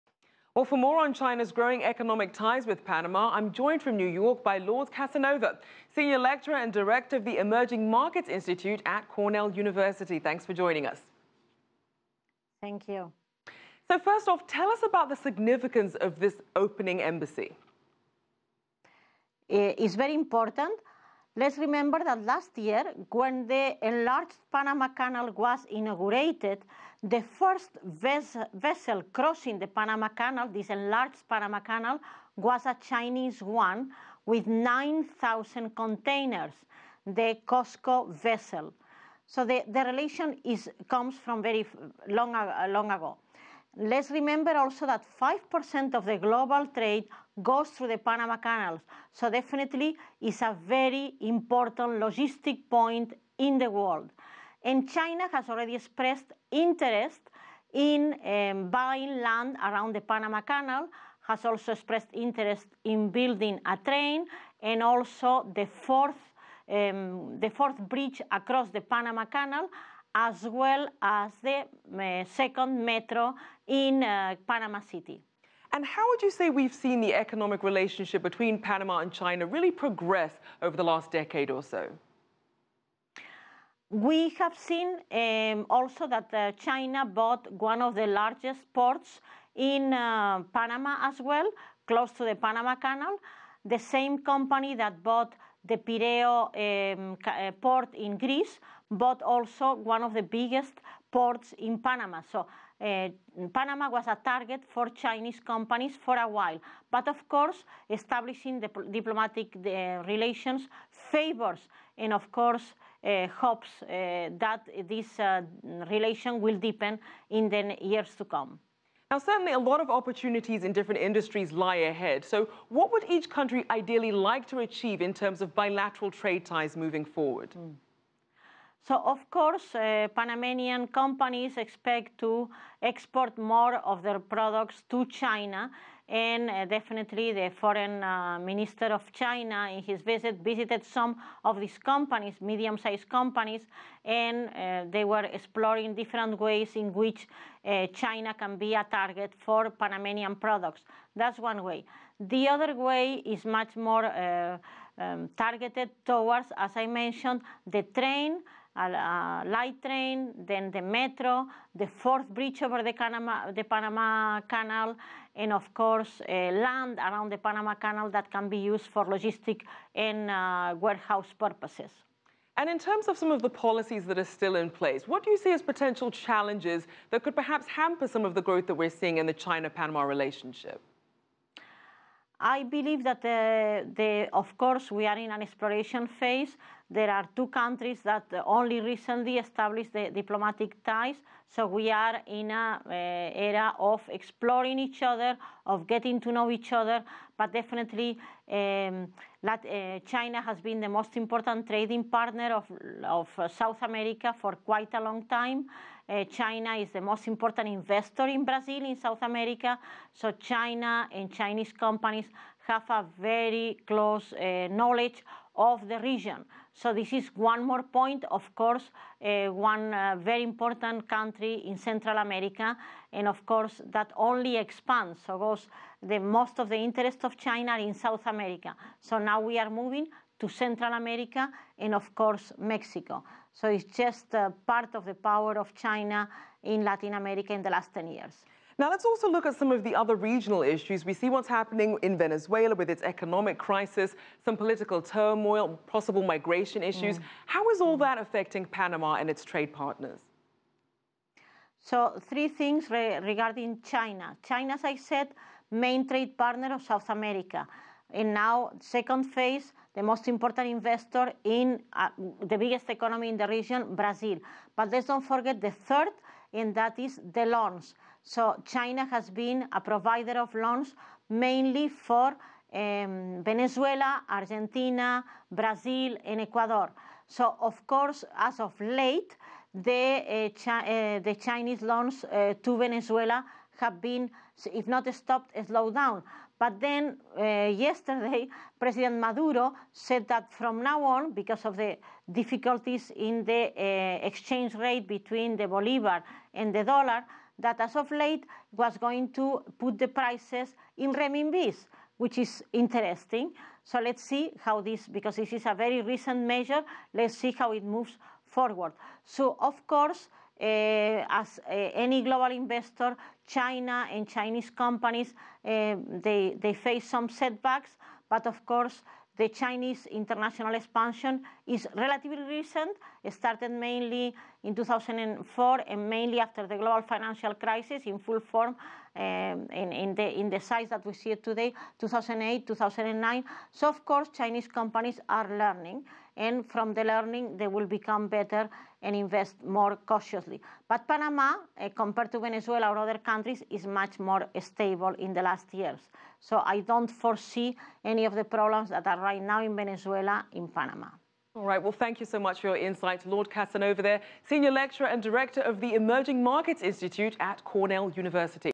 Lourdes Casanova on China's growing economic ties with Panama
CGTN's Rachelle Akuffo spoke with Lourdes Casanova, senior lecturer and director of the Emerging Markets Institute at Cornell University about China's growing economic ties with Panama.